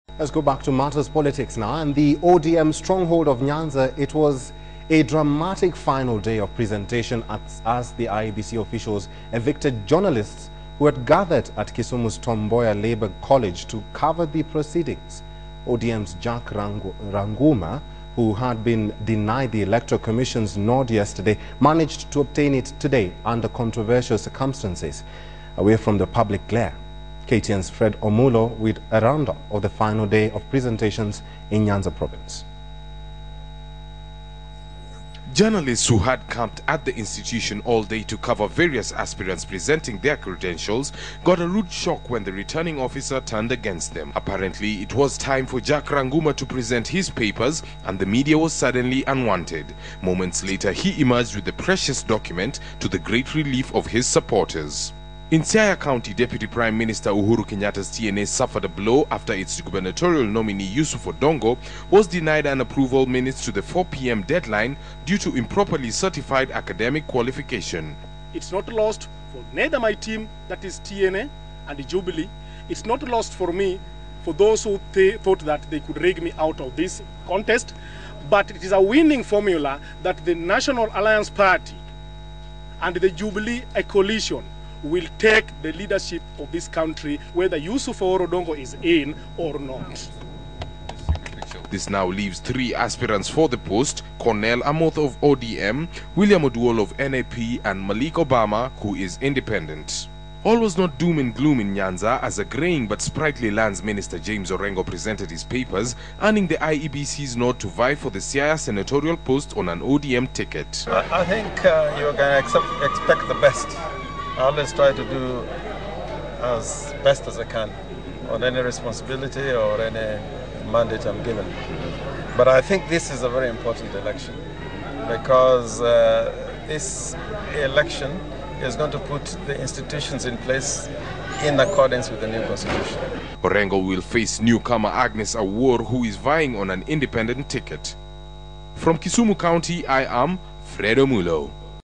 News: Nominations deadline drama in Nyanza
In the odm stronghold of nyanza it was a dramatic final day of presentation as iebc officials evicted journalists who had gathered at kisumu's tom mboya labour college to cover the proceedings. Odm's jack rangumawho had been denied the electoral commissions nod yesterday managed to obtain it today under controversial circumstances, away from the public glare. Ktn's fred omulo with a roundup of the final day of presentations in nyanza province ...